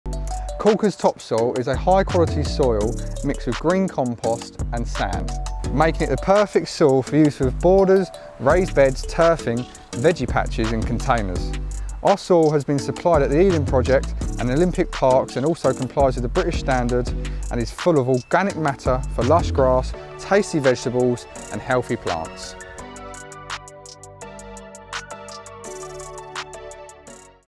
The best quality Topsoil for your garden from Corker!
A Perfect Balance

With a perfectly balanced blend of sandy loam and compost, our topsoil is rich in organic matter and plant nutrients. Whether you’re creating new lawns, laying turf, growing flowers and vegetables, creating raised beds or filling containers, it’s ideal.

What’s more, this topsoil comes with some pretty top-notch endorsement too. In fact, it is the topsoil of choice for both the Eden Project, and for the Olympic Park during construction. Widely used in the landscaping industry by those in the know, our topsoil really is at the top of its game. Which means it’s up to the job and won’t let you down

Sets the Standard

Topsoil for Corker Outdoors complies with the current British Standard BS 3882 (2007) for fertility, freedom from contaminants, as well as soil structure and classification. It has also passed the Contaminated Land Exposure Assessment (CLEA) and met the stringent standards of the National House Building Council.

All that means is that this is a tried and tested topsoil, literally! So, you get a product of excellent quality, and one produced and supplied within the UK to an impeccable standard.